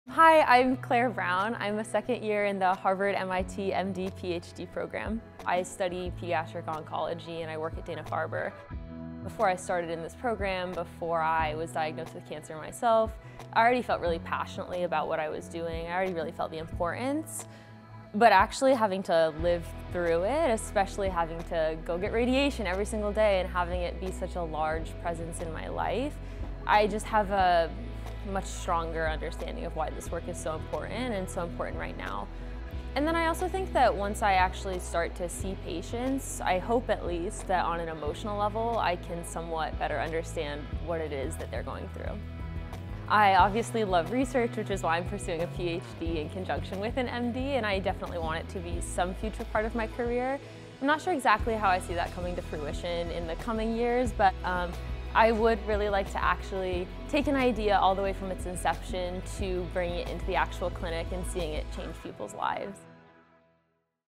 Harvard MD-PhD Student Shares Her Motivation for Cancer Research
A month before starting medical school, Claire Brown was diagnosed with cancer.

The MD-PhD student in the Harvard-MIT Health Sciences and Technology - HST Program was already passionate about cancer research. Living through the disease underscored the importance of the work.

“I would really like to actually take an idea all the way from its inception to bringing it into the actual clinic and seeing it change people’s lives,” Brown said.

Brown looks forward to spending more time in the hospital as an opportunity to discover where she is best suited in clinical practice.